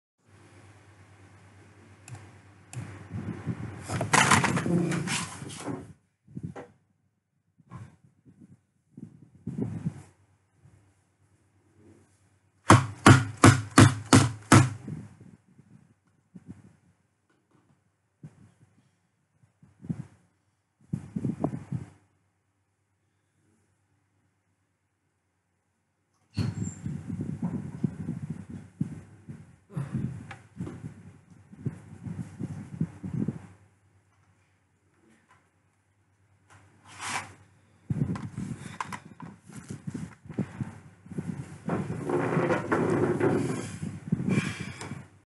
Cockroach shock
browsing thru reddit when cock roach appeared
gotta clean my room soon, its getting so messy that roaches are nesting here..